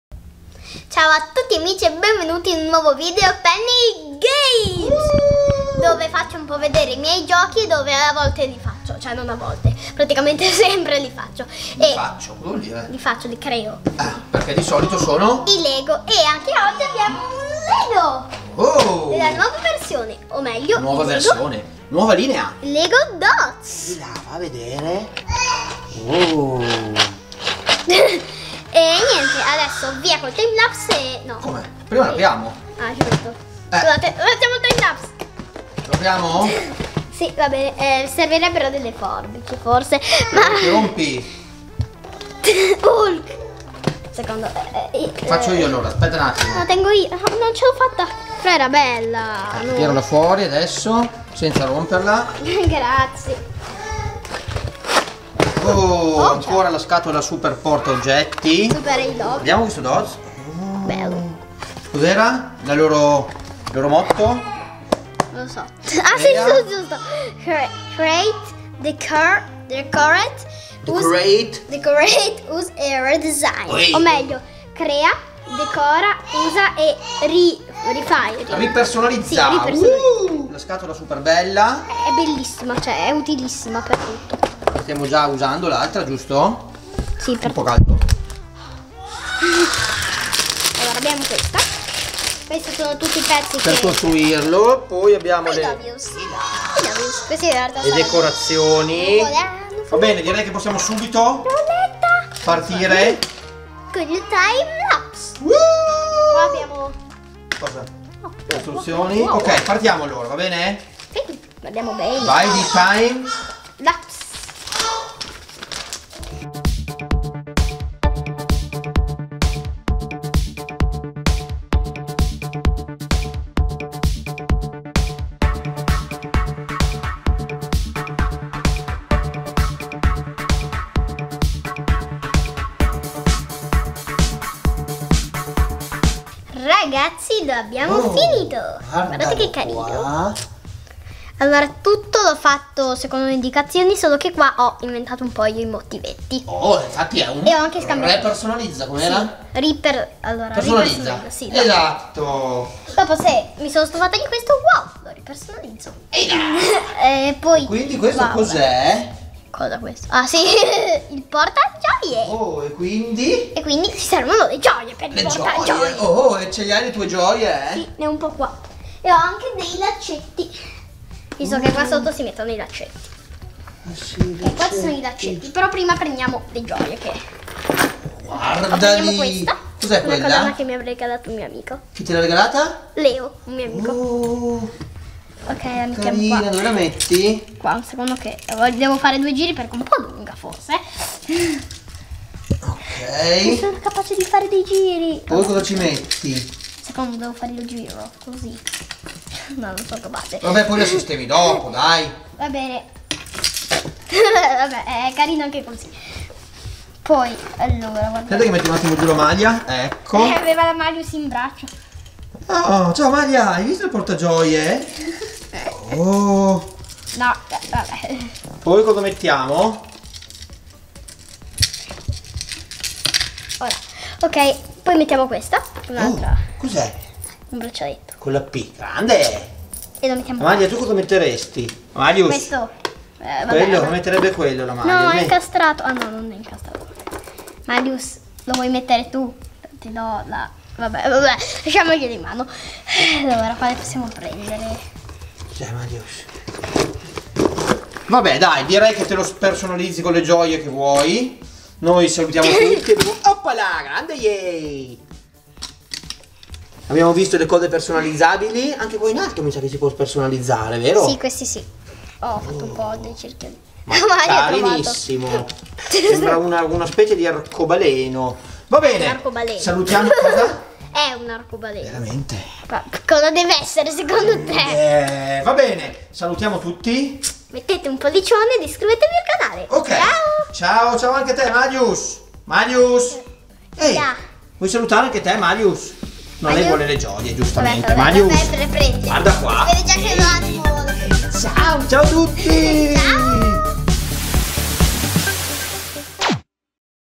LEGO DOTS PORTAGIOIE | Recensione Penny Games #24
La Penny torna, dopo un pò di tempo, alla sua Rubrica Penny Games, nella quale vi parla dei giochi che ama di più.

Come spesso capita sono dei LEGO.
Nello specifico i nuovi Lego Dots. Set di recente uscita dove i bimbi possono personalizzare la loro creazione, qualche settimana fa vi ha parlato dei Dots Portafoto, questa volta costruirà un tenerissimo Portagioie.

Ecco il video della Penny, dove ve li mostra 😜